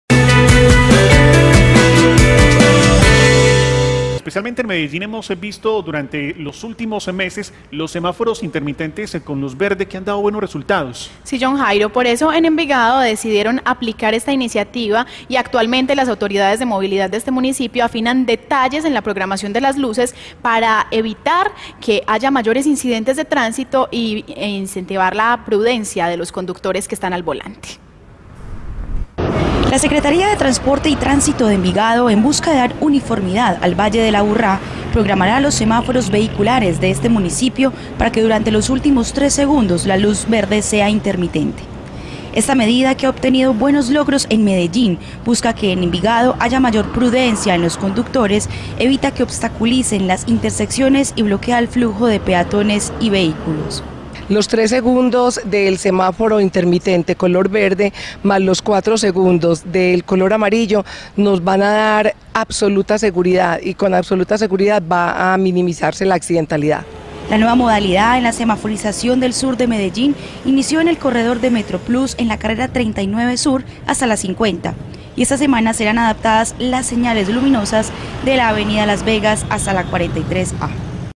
Envigado adoptó la medida de semáforos con luz verde intermitente
El Municipio de Envigado adoptó la medida de semáforos con luz verde intermitente que buenos logros ha obtenido en Medellín. La Secretaría de Tránsito y Movilidad del Municipio finalizará la nueva programación de las luces esta semana, con el fin de incentivar la prudencia de los conductores y minimizar accidentes vehiculares.